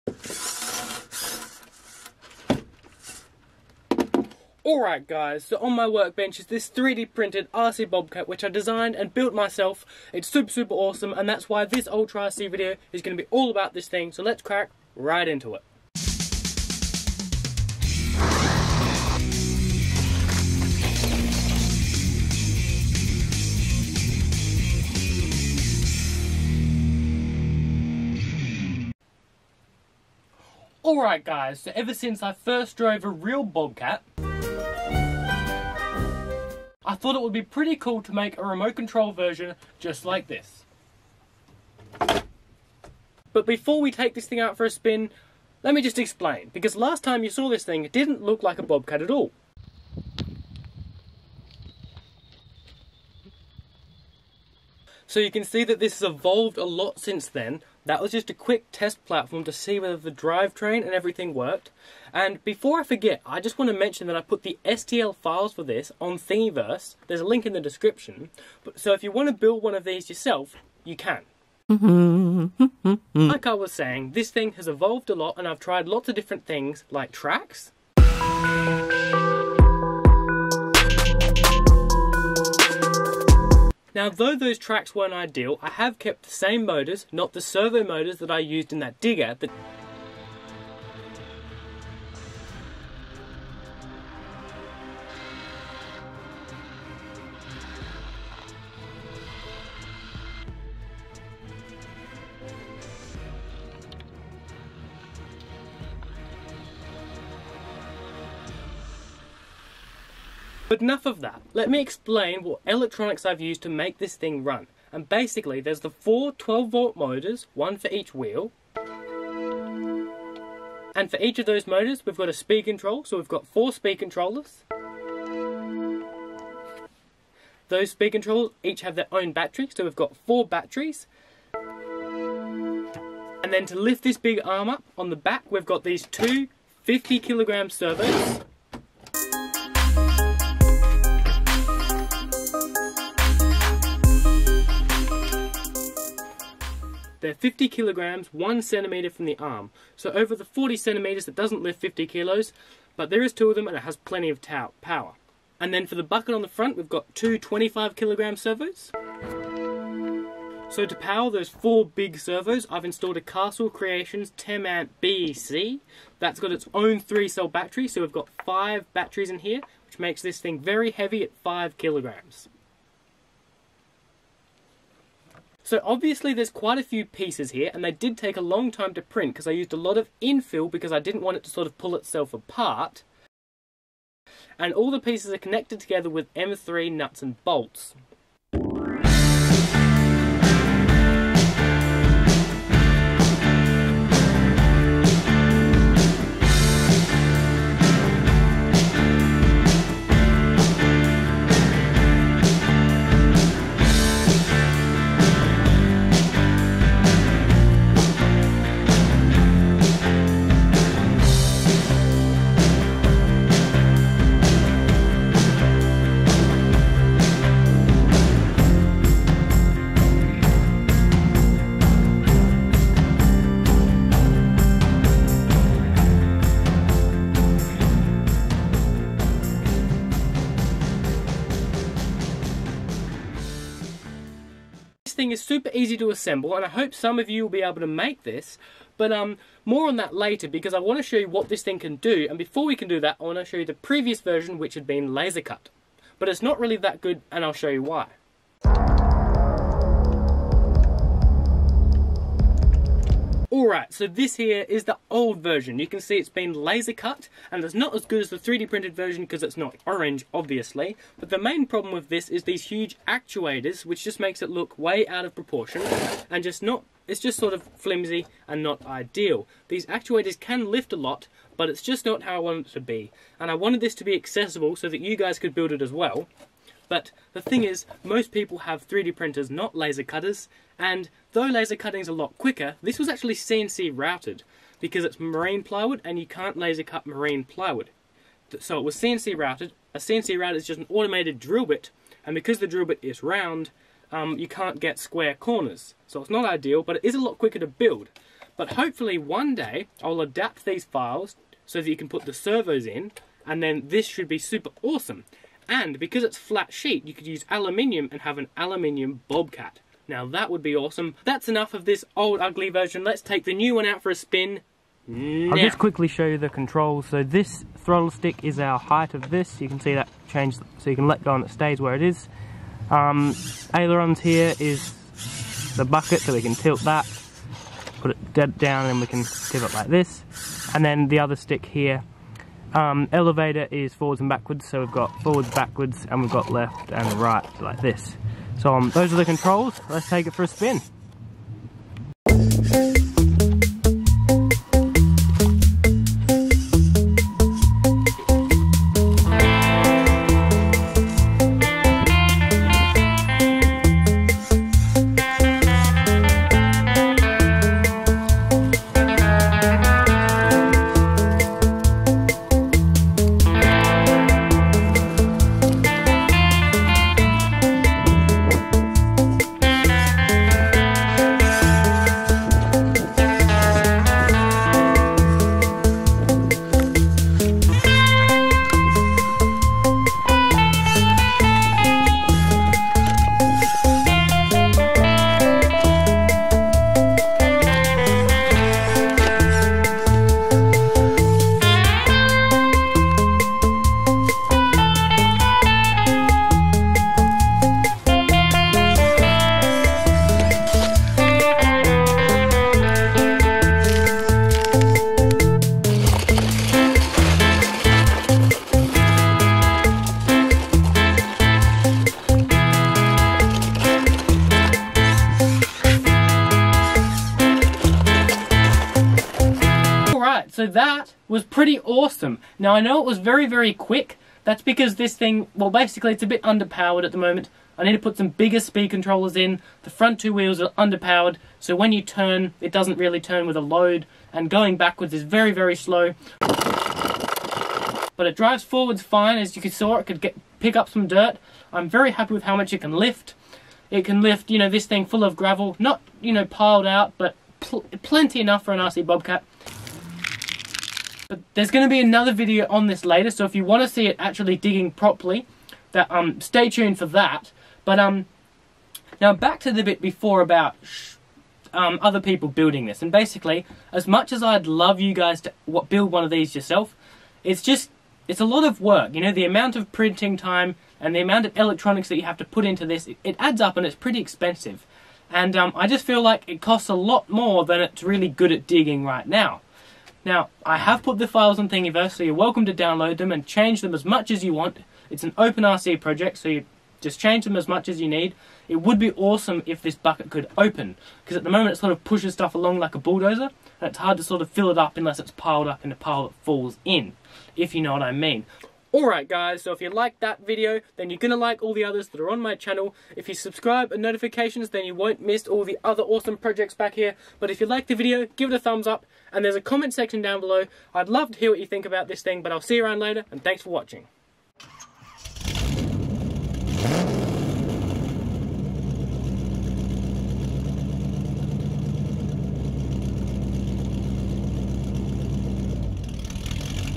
3D PRINTED RC BOBCAT //ULTRA RC
I made this 3d printed rc bobcat and this awesome ultra rc video is all about it so i hope you like this video.

This is a link to the stl files on thingiverse!

i would love to have some feedback on the designs if anyone has any ideas
there is lots of information on the video so check that out, but basically there is not many options out there for rc bobcat's and i think they are the ideal digging machine, so i designed my own!!!
It is pretty awesome but not to cheap.

This has 3D printed wheels on it, but the motors use an adapter which has a 12mm hex so any rc wheels will fit. I designed the 3D printed ones because of the deep wheel insert and the weight of the bobcat is pretty heavy and needed more support for the skid steering.
you need the one with 6mm shaft

This makes for a very strong drive train with no weak driveshafts and cogs

This is the bucket servo

This is the esc i'm going to install to give it more power!!!

and these are the motors

If anyone does make one of these i would love to see some pictures!

Intro music!

BY FOR NOW!!!